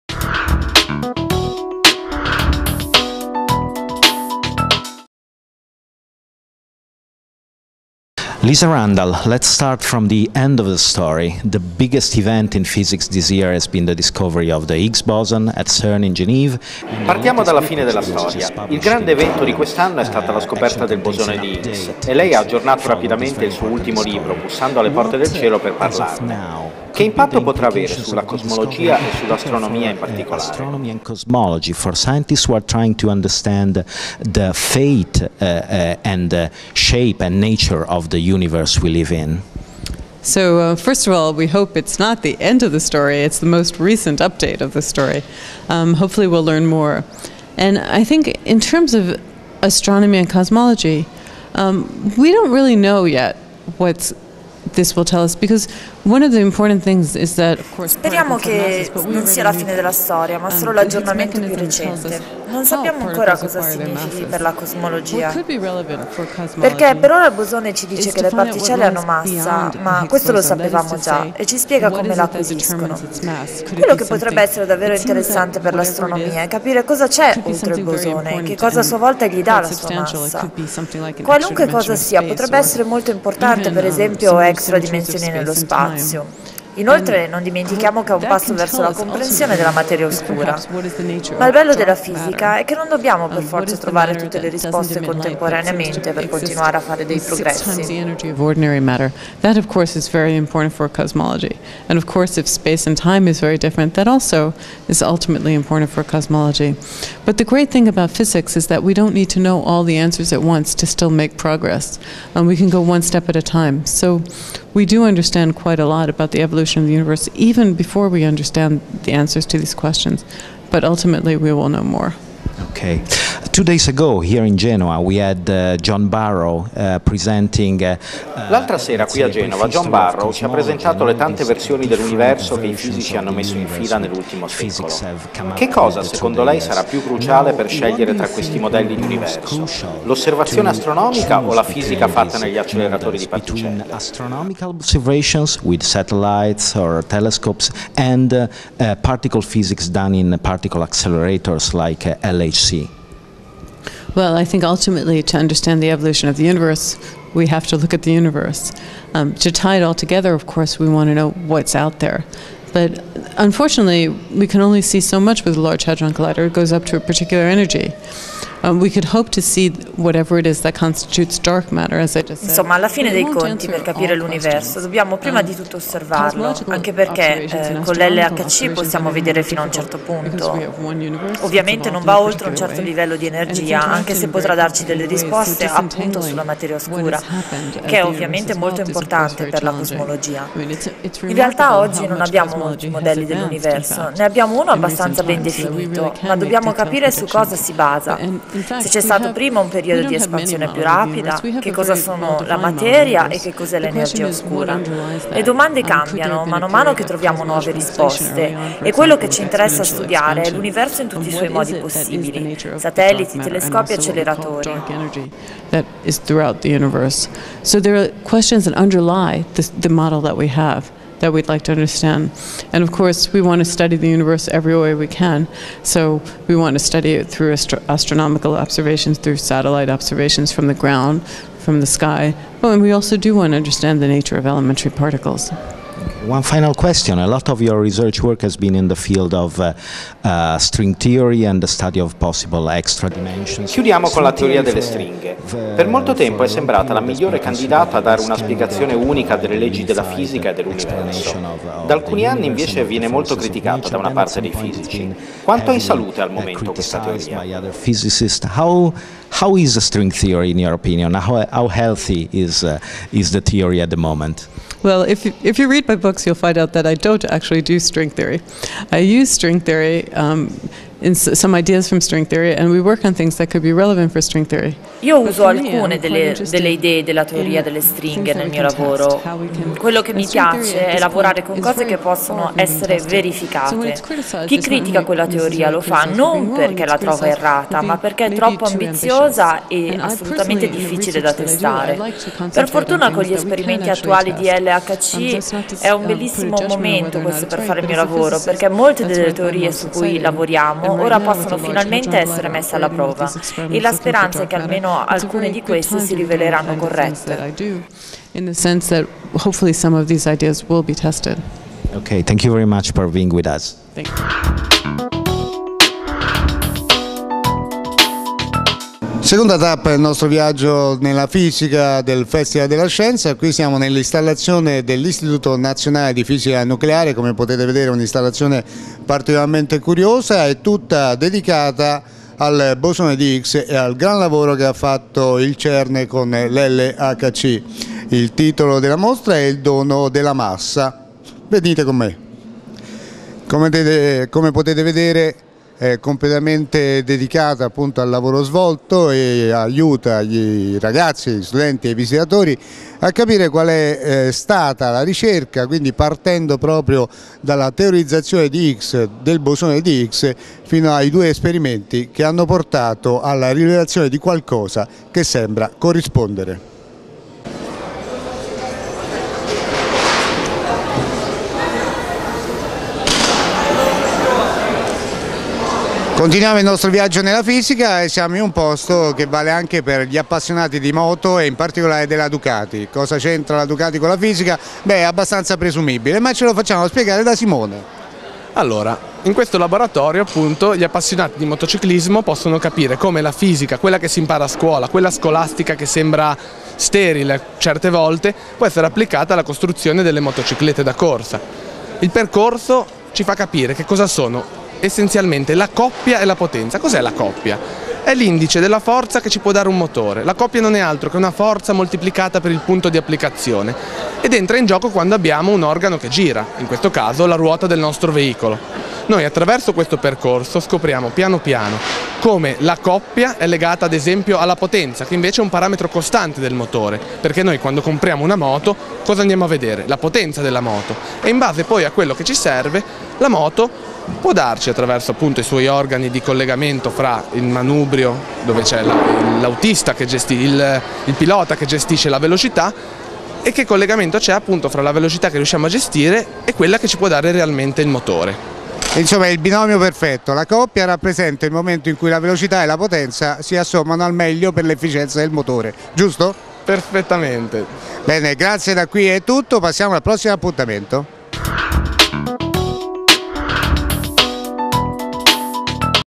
Bussando alle porte di Higgs
Il Festival della Scienza di Genova arriva alla quarta giornata, ed è l'occasione per altre incursioni nei meandri della fisica. Varchiamo le soglie del cielo con Lisa Randall --  fisica e cosmologa statunitense e autrice, appunto, di "Bussando alle porte del cielo" -- per capire che impatto potrà avere la scoperta del bosone di Higgs sulla scienza dell'universo. E sempre rimanendo con gli occhi puntati sul bosone più famoso di sempre, visitiamo la mostra dell'INFN a esso dedicata, "Il dono della massa".

Servizi e riprese di Francesco Rea e Nicola Nosengo (Media INAF)